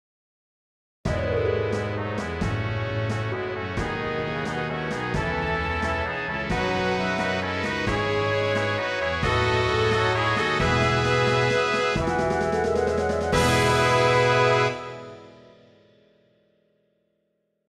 cpe final logo promise
This took me 3 days to make, and I consider this to be my favorite logo.